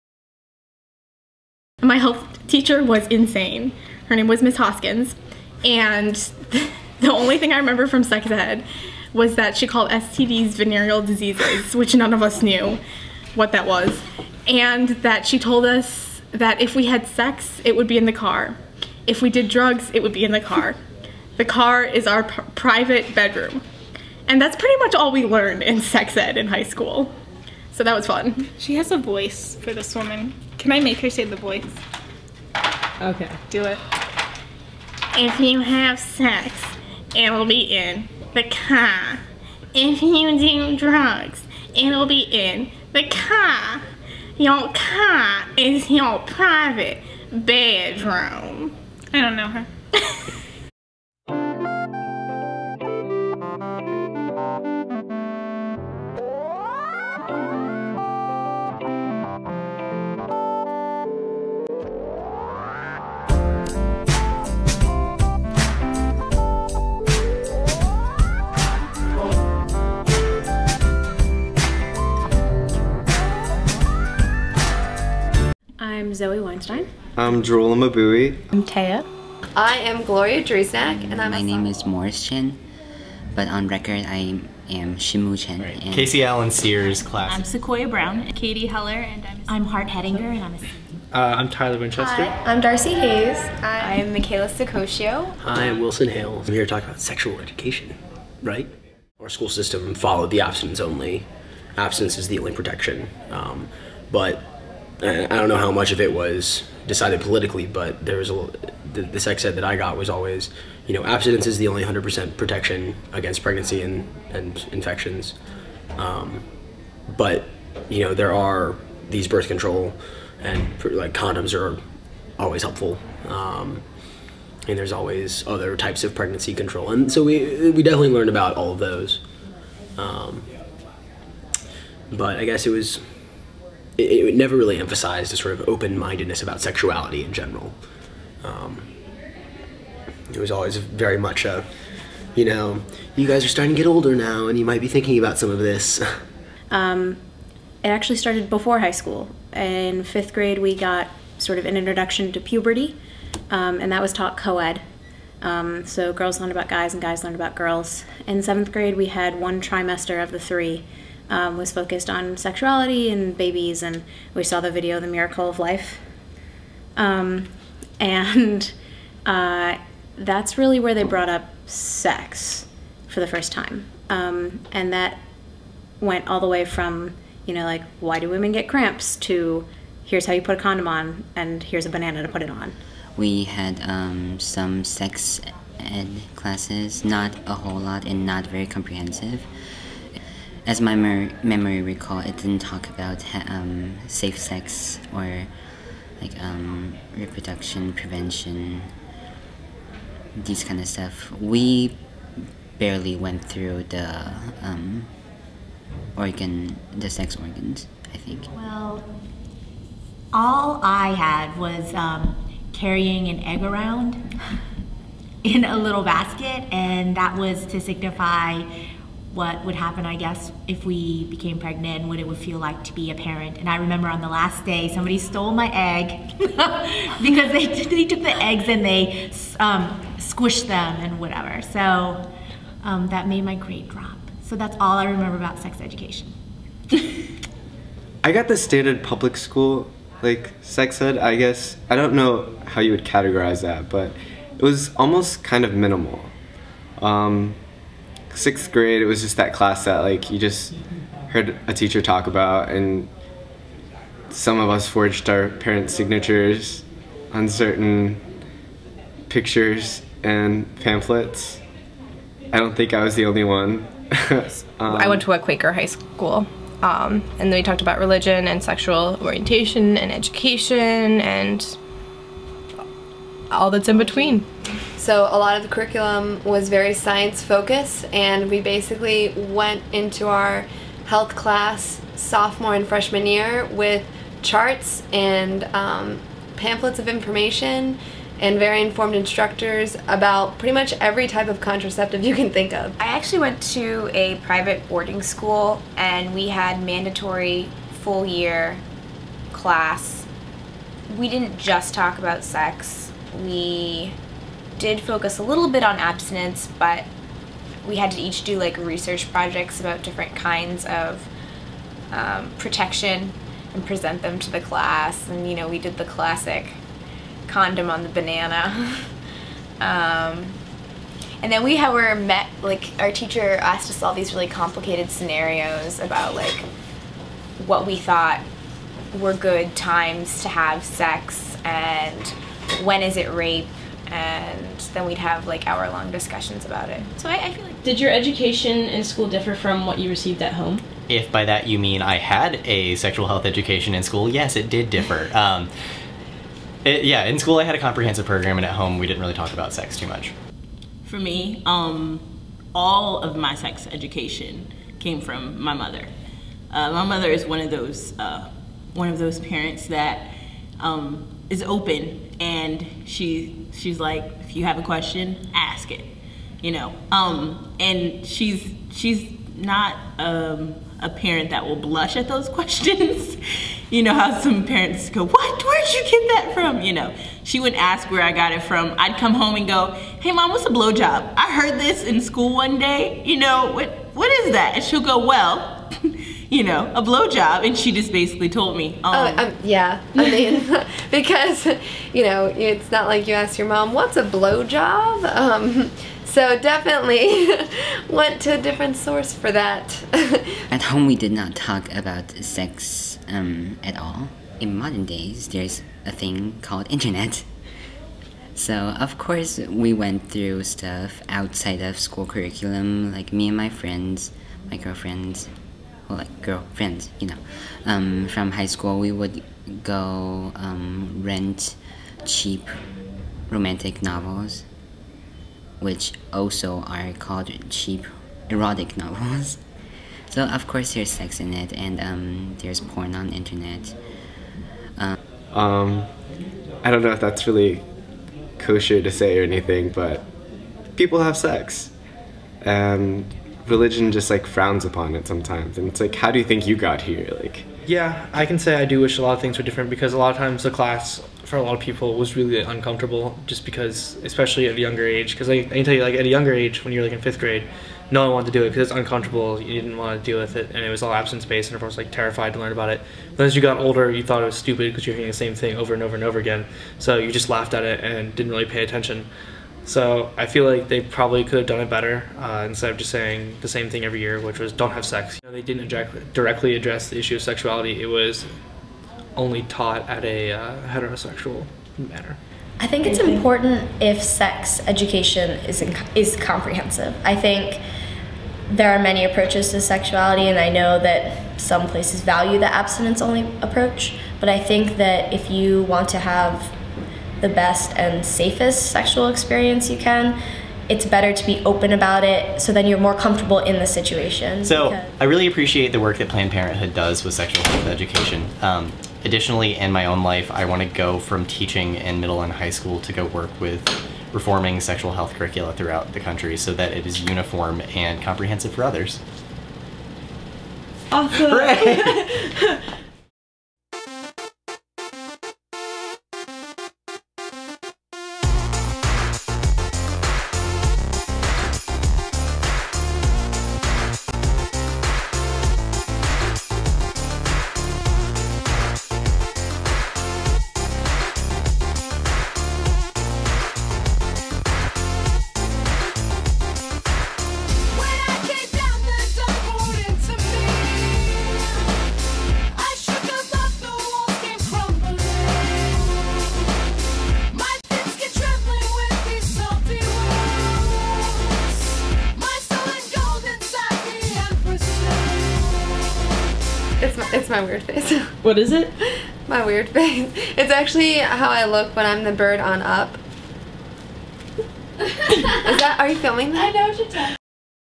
School of Sex Education
Completed for an Independent Study, I interviewed several of my fellow peers at the College of William and Mary about their "sex ed" background. These students are just a small sampling of the school and are from many different walks of life, states, and even countries. I hope you find this to be interesting and that you may develop an appreciation for such an education, and that you begin to wonder about the inequalities that are present in sexual education.

I'll probably think of something else to add here.

(Get educated!)